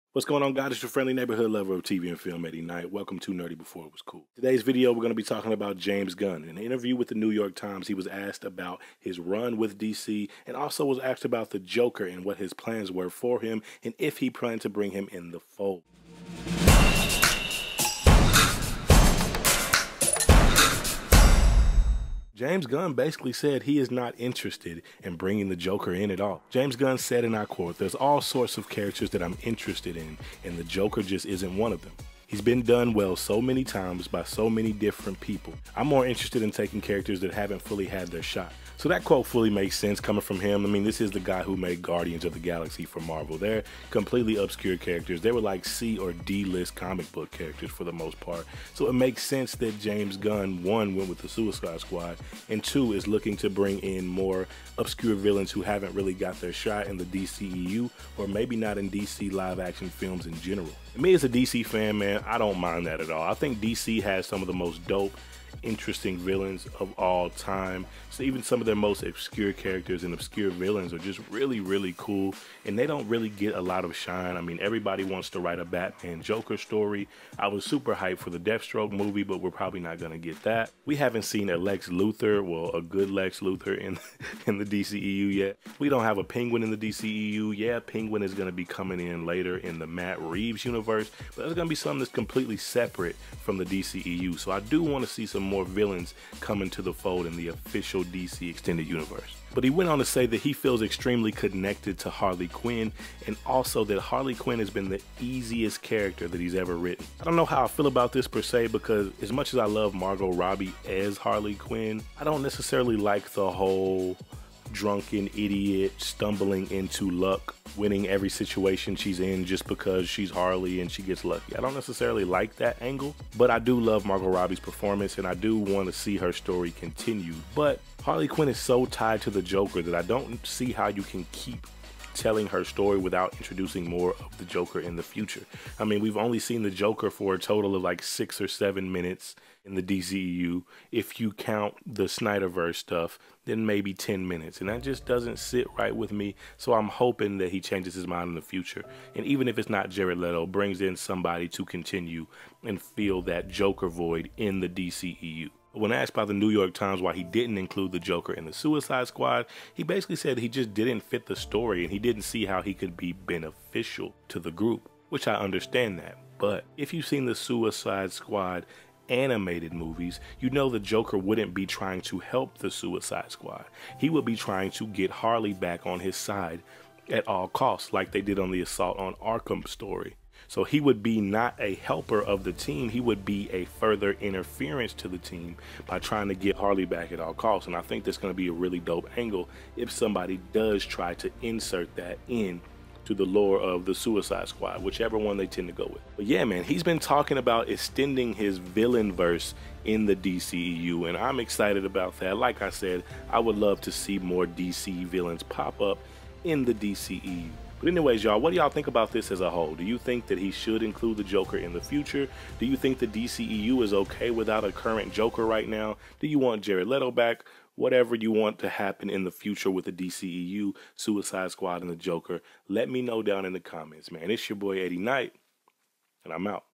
Why James Gunn Is NOT Interested In The Joker | DCEU News
James Gunn is still at the helms of Harley Quinn & The Suicide Squad. When asked about the Joker's involvement in future stories the director says he "ins't interested" LETS TALK ABOUT IT!